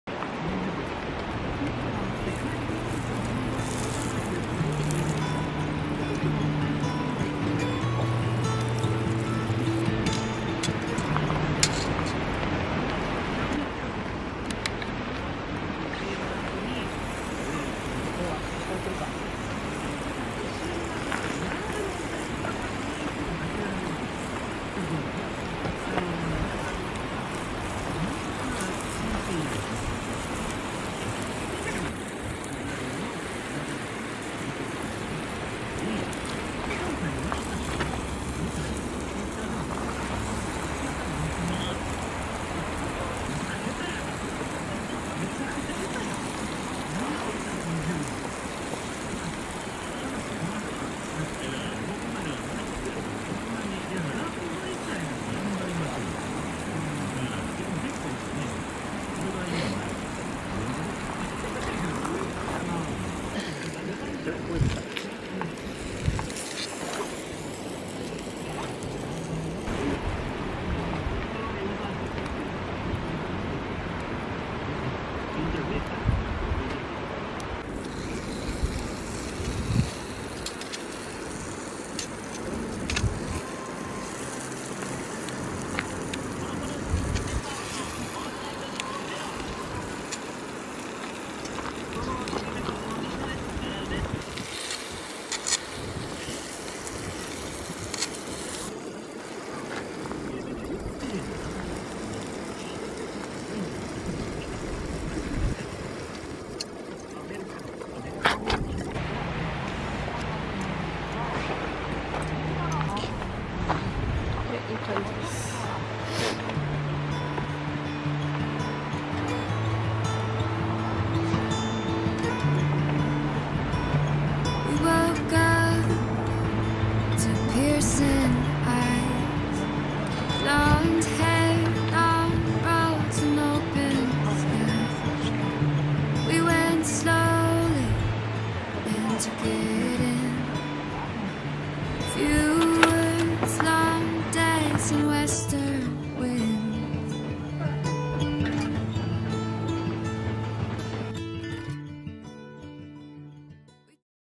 【キャンプ動画】焚火で黒毛和牛の厚切りステーキを焼く
こんにちは、Nicole＆圭史です。

初めてのキャンプ動画になります！！！！謎にテンション上がります。笑
今回の動画は「焚火で黒毛和牛の分厚いステーキを焼く」です。
自然と寒さを感じながら、熱々の黒毛和牛本来の味で
そのまま食べるっていう幸せなひと時でした。

焚火の土台も圭史が自ら作ったりと
動画の撮影にあたるまで過程も良い時間でした。良い思い出。

このページ、この動画をご覧いただき

今後も料理動画やキャンプ動画などなど
載せていくので引き続きよろしくお願いします。